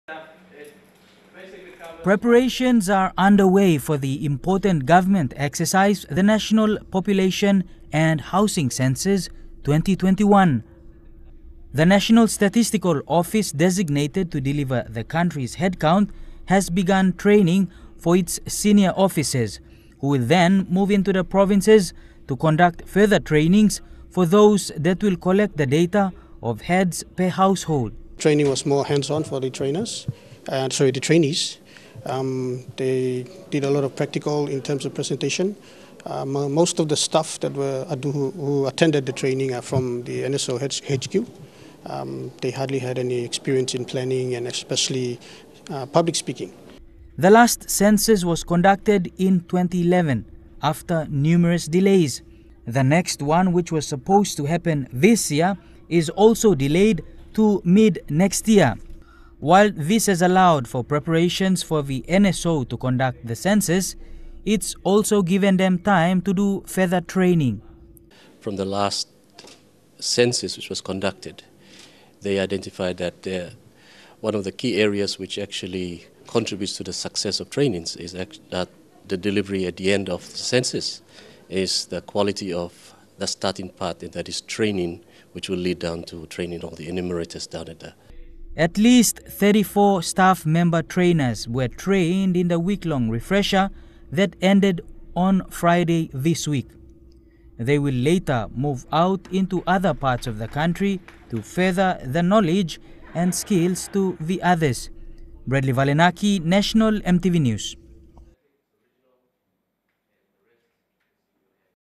National Statistical Office Trains Staff ahead of Census 2021
The National Statistical Office has commenced training its senior staff to prepare them, for the national census next year.

Acting Census Director Michael Kaivepa says this is the first step in ensuring that the exercise is done with precision and that the country's population is recorded properly.